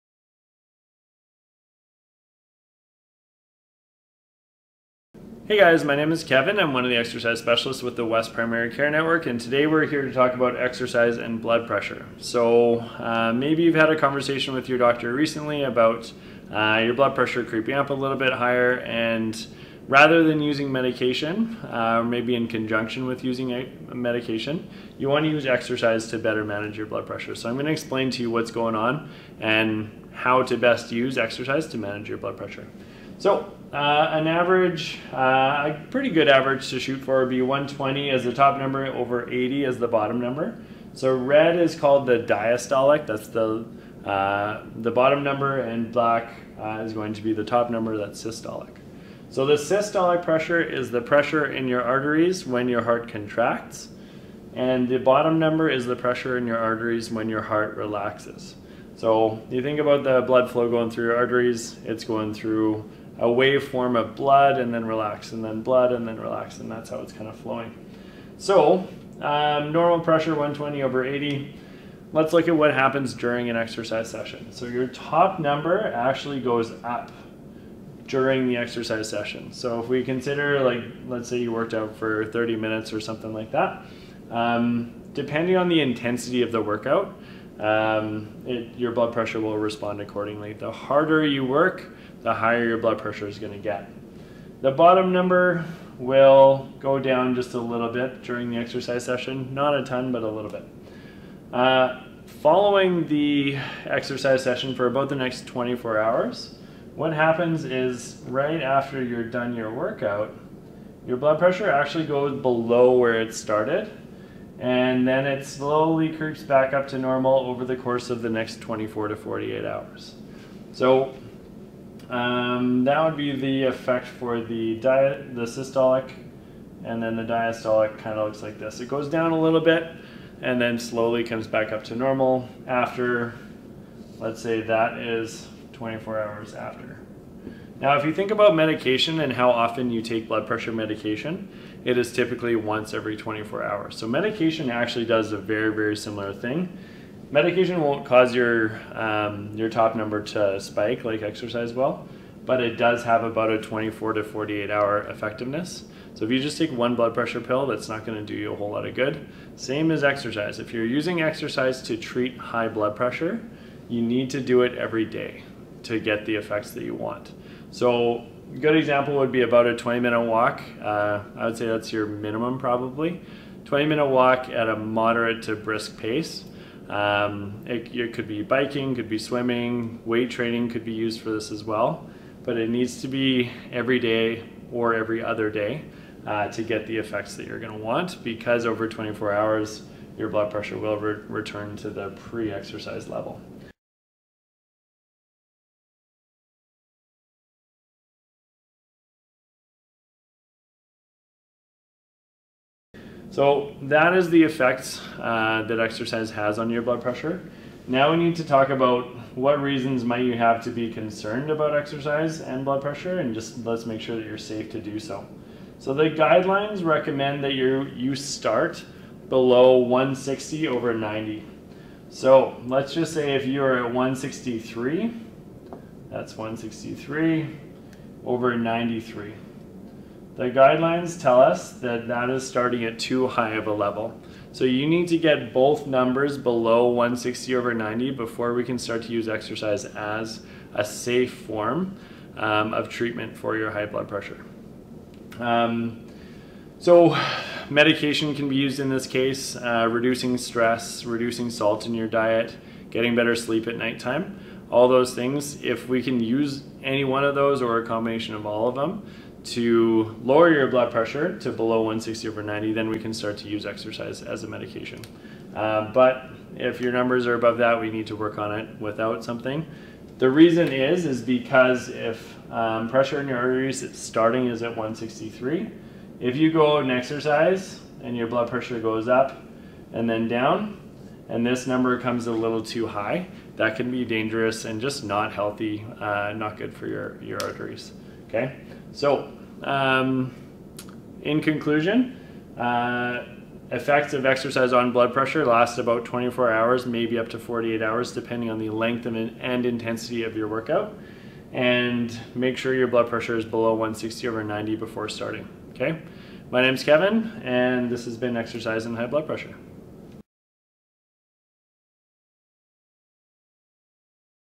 High Blood Pressure & Exercise
Exercise Specialist Kevin with the Edmonton West Primary Care Network talks about how  exercise can help you to manage hypertension or high blood pressure. He describes the mechanisms behind how it can help, how we use exercise as medicine, and the guidelines around safely exercising with high blood pressure.